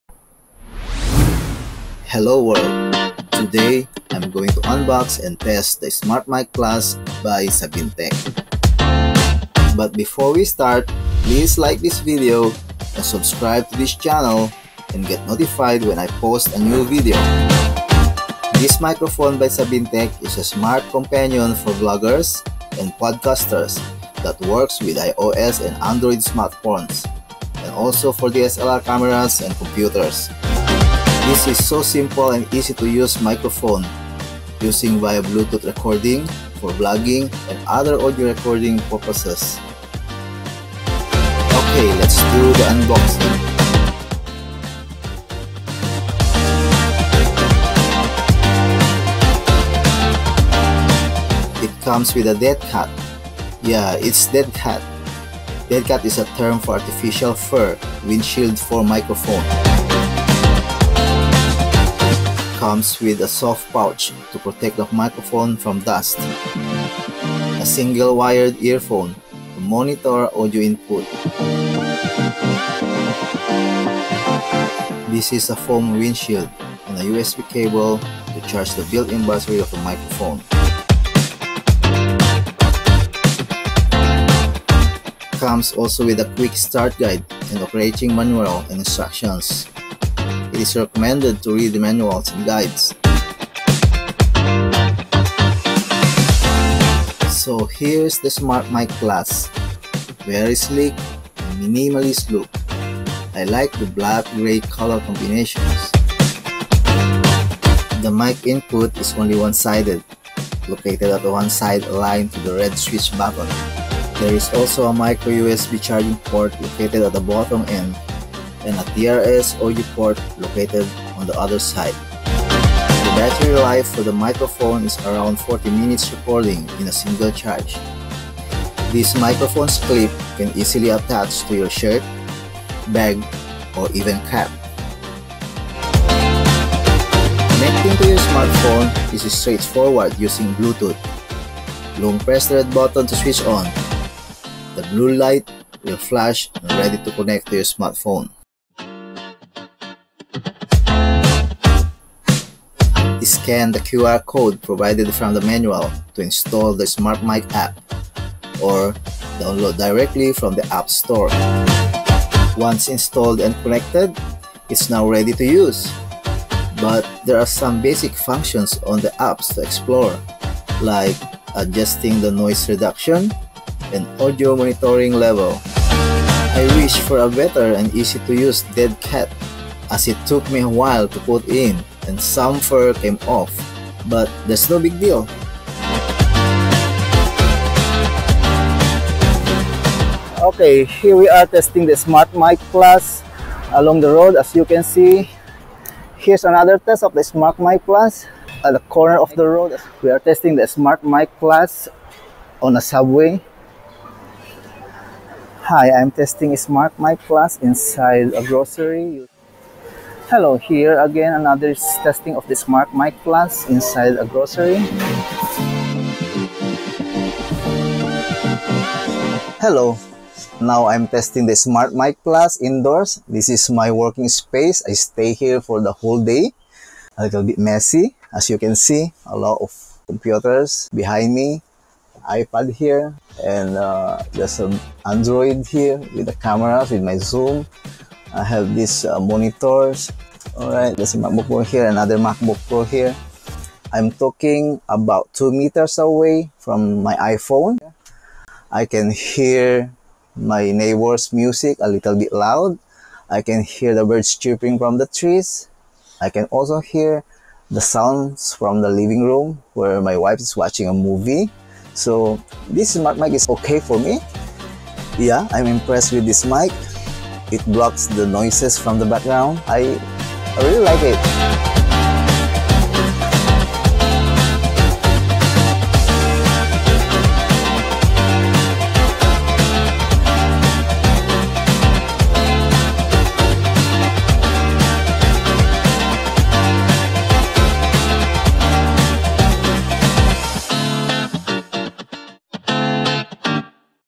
SmartMike+ Unboxing and Quick Testing
This is an unboxing and basic testing of the SmartMike+ from SabineTek,  This is NOT a product review.

This bluetooth microphone. is  so simple and easy to use without having wires wrapped around your neck and having problems with connectivity. This is good for vloggers and some sort of audio recording. It will also work with DSLR cameras and computer's sound card.

There are pros and cons about this microphone which not mentioned at the video as I said, this is not a product review.

I will be testing more of its capabilities and post some results here or maybe another video explainer.